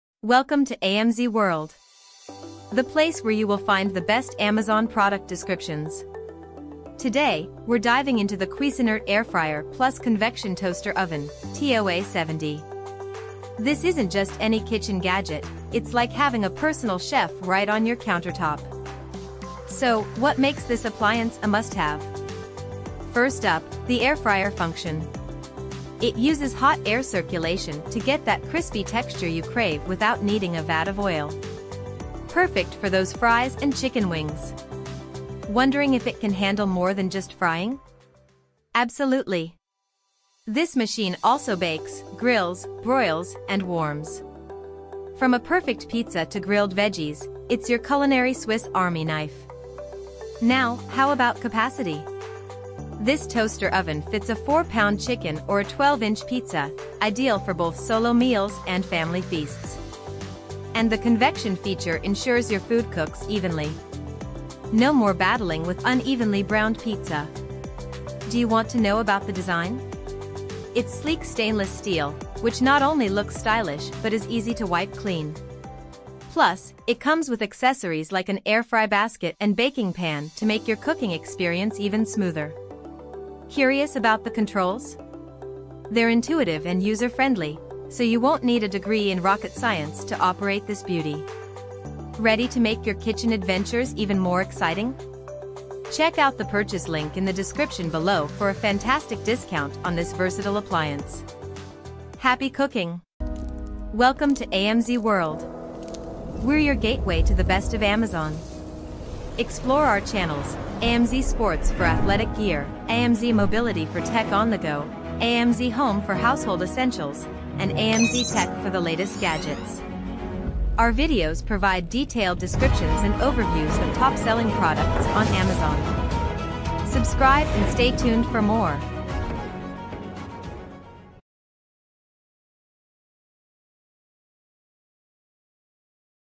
Describing Cuisinart Air Fryer + Convection Toaster Oven, 8-1, Amazon
The Cuisinart Air Fryer + Convection Toaster Oven (TOA-70) is a versatile kitchen appliance. It combines the functions of an air fryer and a convection toaster oven, allowing you to bake, grill, broil, and warm food. Here are some key features of this model:

Air Fryer Function: It uses hot air circulation to cook food with little to no oil, giving you crispy results similar to deep frying but with less fat.

Convection Oven: The convection feature ensures even cooking by circulating hot air around the food.

Versatile Cooking Options: In addition to air frying and convection baking, it offers options for baking, grilling, broiling, and warming, making it a multifunctional appliance.

Capacity: The 8-in-1 functionality means you can use it for a wide range of cooking tasks, from baking a small pizza to grilling vegetables.

Design: The stainless steel finish not only looks sleek but also is easy to clean. The size is typically compact enough to fit on most countertops without taking up too much space.

Controls: It usually features intuitive controls and preset functions, allowing for easy adjustments and precise cooking.

Accessories: Often comes with accessories like an air fry basket, baking pan, and rack, which enhance its versatility.

This model is well-regarded for its ability to perform multiple cooking functions, making it a great addition to any kitchen.

Welcome to AMZ World! The place where you will find the best Amazon product descriptions! Today, we’re diving into the Cuisinart Air Fryer + Convection Toaster Oven, TOA-70. This isn't just any kitchen gadget—it's like having a personal chef right on your countertop!

So, what makes this appliance a must-have? First up, the air fryer function. It uses hot air circulation to get that crispy texture you crave without needing a vat of oil. Perfect for those fries and chicken wings! Wondering if it can handle more than just frying? Absolutely! This machine also bakes, grills, broils, and warms. From a perfect pizza to grilled veggies, it’s your culinary Swiss Army knife.

Now, how about capacity? This toaster oven fits a 4-pound chicken or a 12-inch pizza—ideal for both solo meals and family feasts. And the convection feature ensures your food cooks evenly. No more battling with unevenly browned pizza!

Do you want to know about the design? It’s sleek stainless steel, which not only looks stylish but is easy to wipe clean. Plus, it comes with accessories like an air fry basket and baking pan to make your cooking experience even smoother.

Curious about the controls? They’re intuitive and user-friendly, so you won’t need a degree in rocket science to operate this beauty.

Ready to make your kitchen adventures even more exciting? Happy cooking!